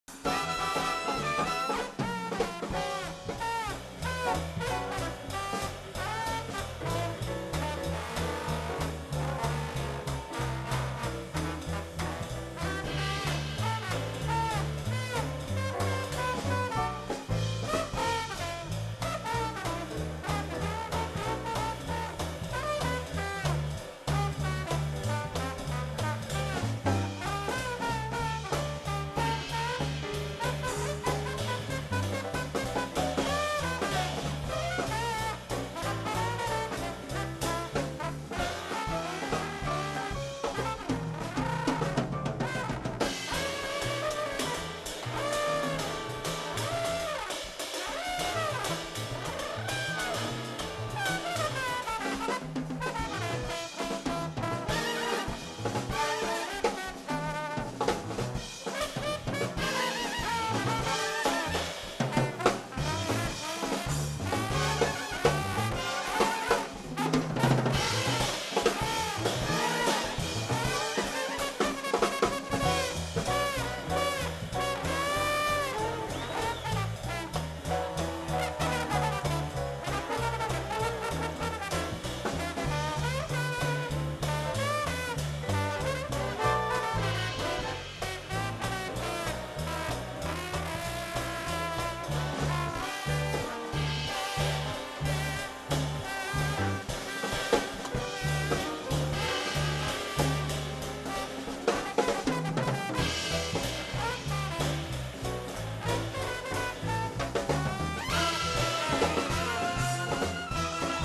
Memorable Jazz Nativity Moments: King Wycliffe Gordon
Performed Live @ B.B. King Dec 22, 2011
King Wycliffe Gordon presents his gift of dance